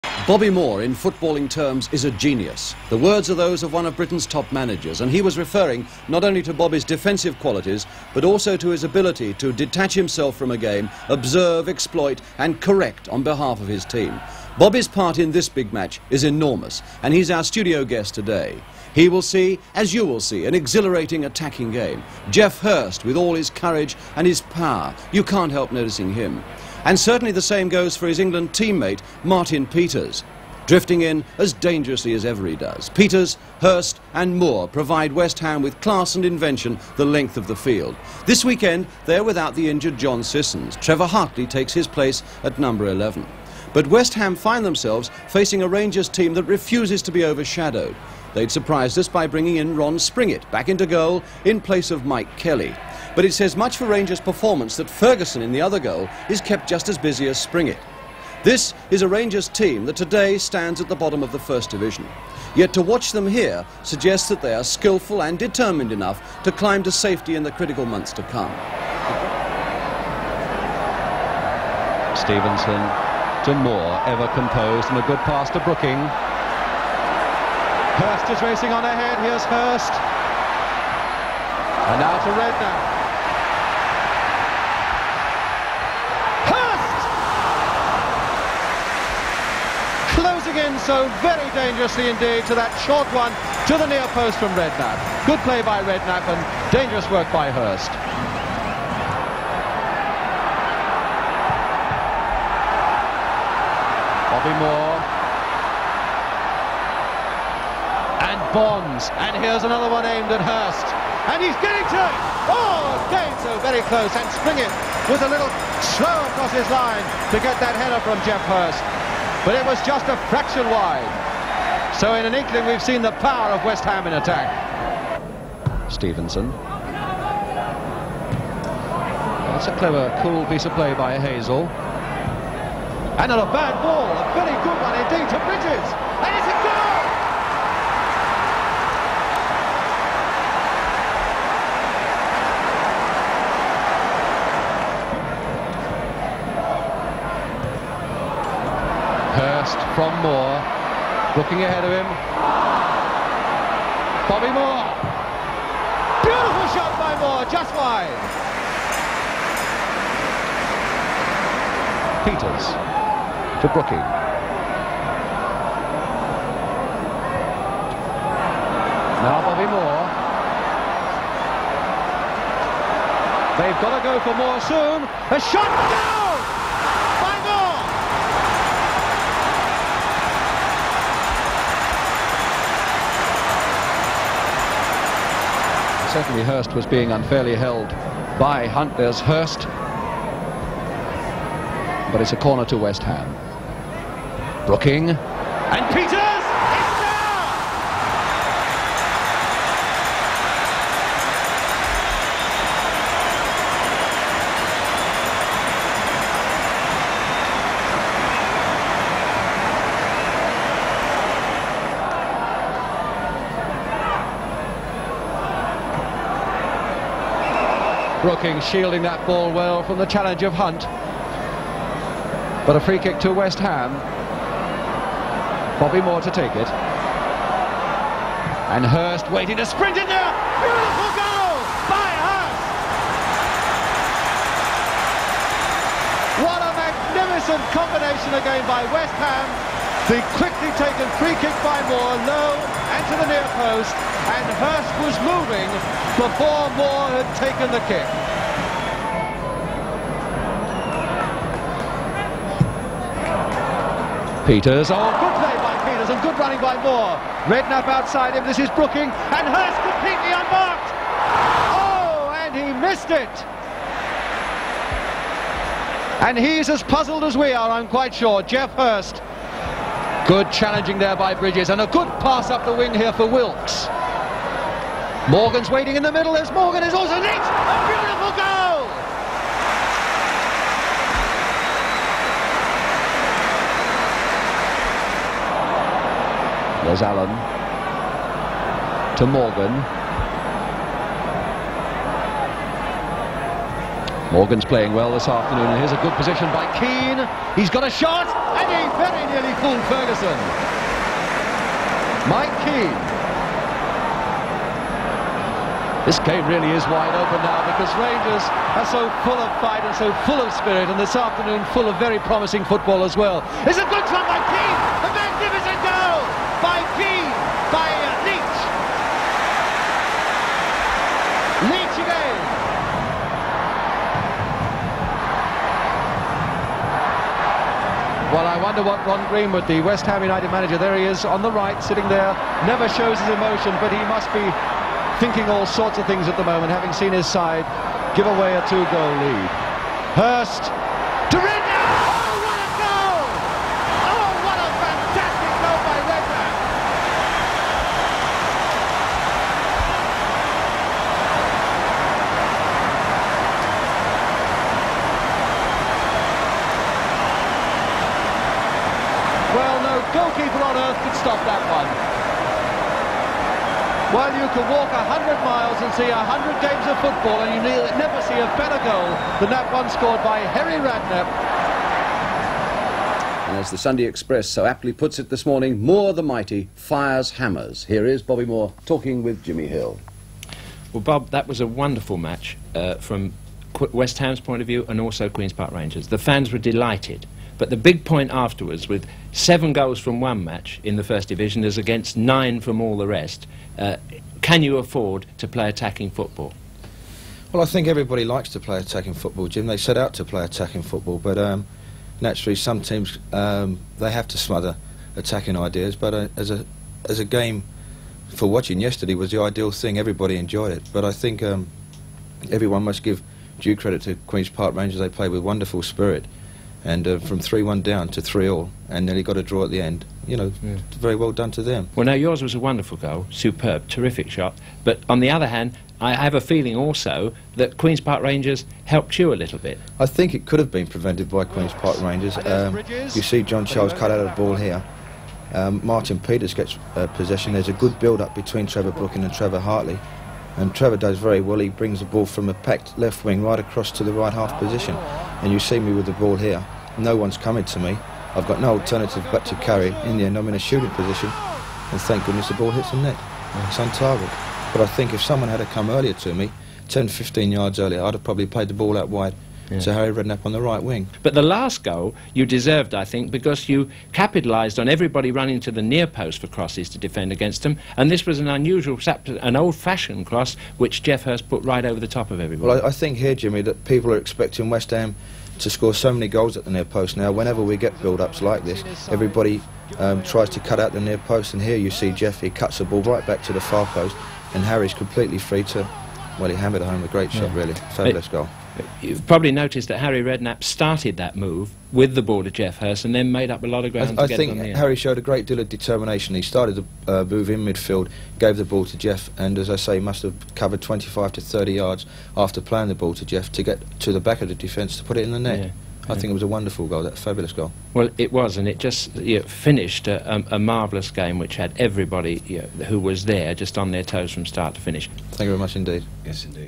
Football: 1968/69: Division 1: West Ham United vs Queens Park Rangers
Highlights of the 1968/69 Division 1 game, West Ham United vs Queens Park Rangers.
Date: Saturday 2nd November 1968.
Venue: Boleyn Ground, London.